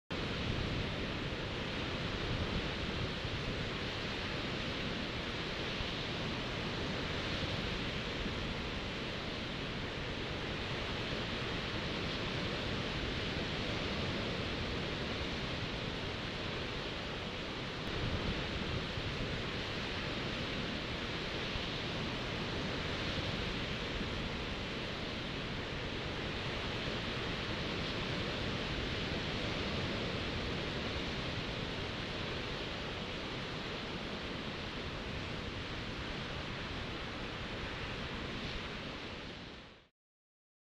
EPIC Fail Photographer
Sometimes a photographer needs to get a better angle. I am so glad I was recording a time lapse sunset with the Go Pro. What you couldn't see were the rocks, rebar, a cement slab and a snow fence pole protruding from the sand that the photographer barely missed on the way down.